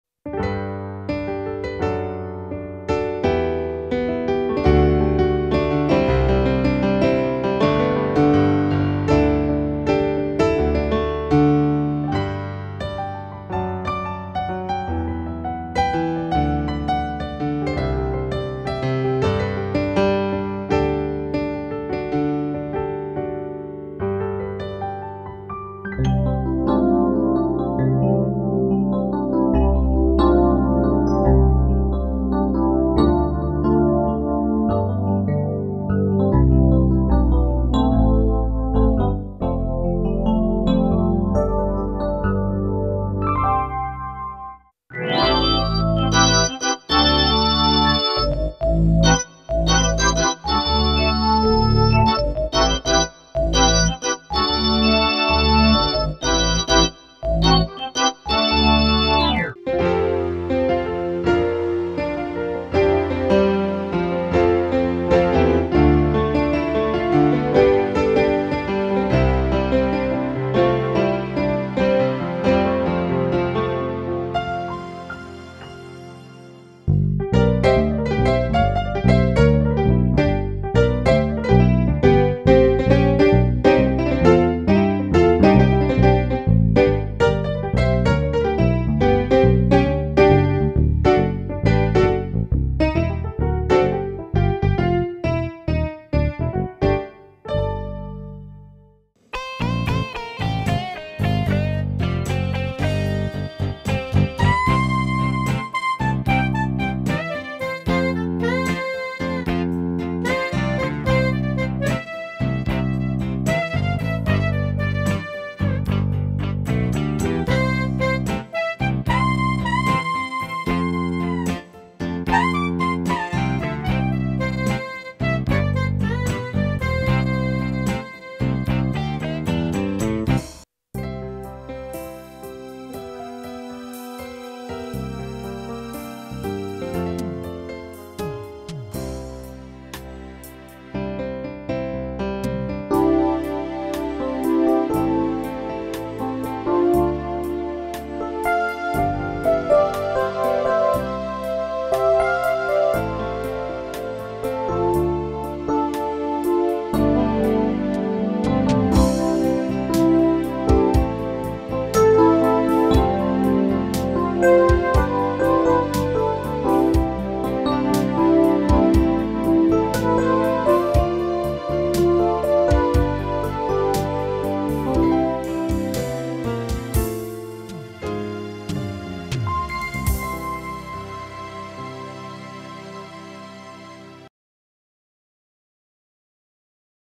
Artesia PE-88 el-piano
Slankt el-piano med fullt klaviatur
88 semi-vektede tangenter
64 toners polyfoni
Med inngang til mikrofon